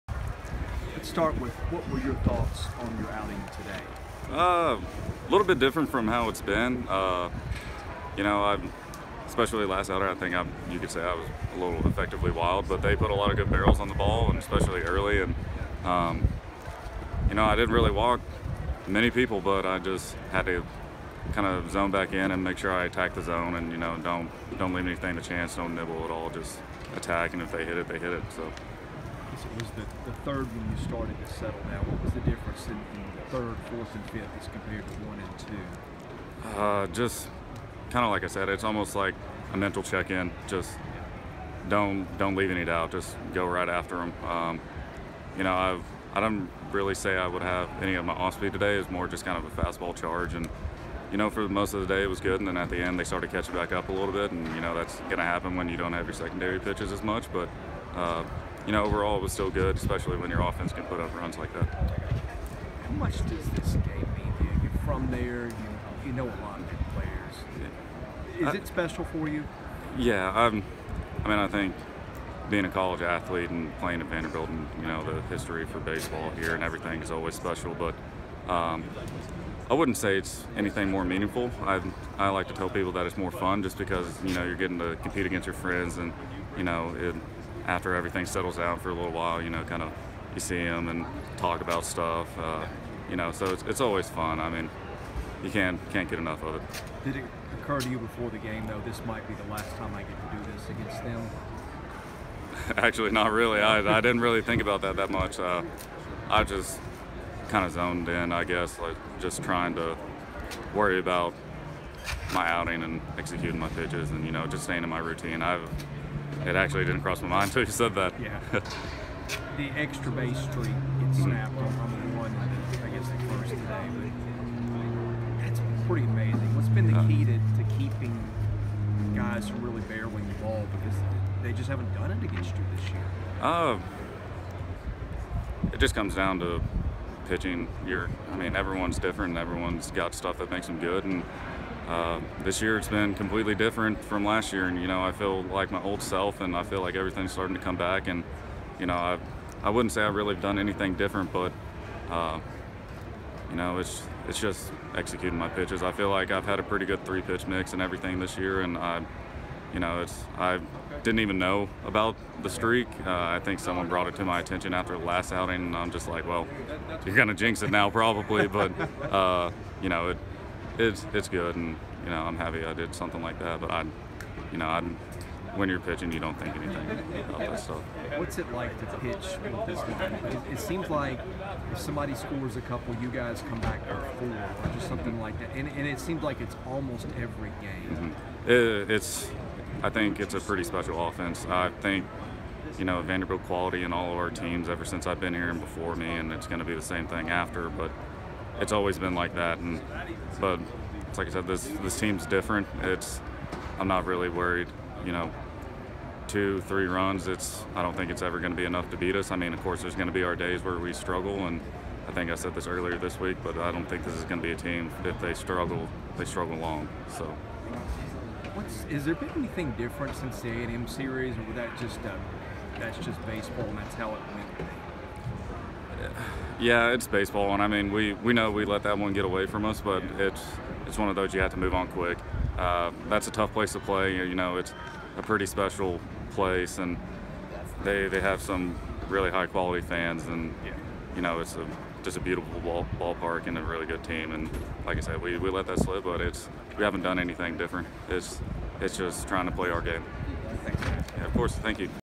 Patrick Raby, March 30
Vanderbilt starting pitcher Patrick Raby talks about a 10-4 win over Tennessee.